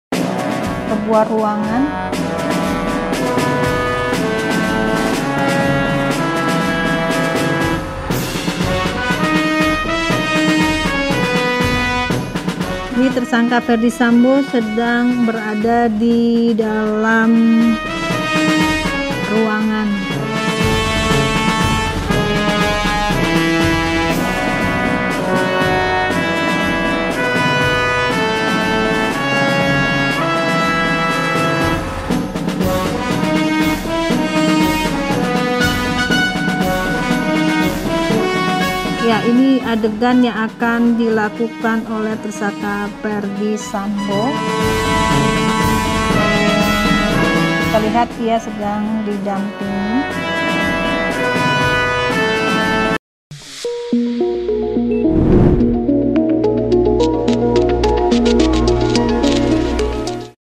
FERDY SAMBO MEMAKAI BAJU TAHANAN SAAT REKONSTRUKSI DI DUREN TIGA
Ferdy Sambo memakai baju orange tahanan saat Rekontruksi Pembunuhan Brigadir J di Rumahnya di jalan Saguling, duren tiga Jakarta Selatan, Selasa (30/8/).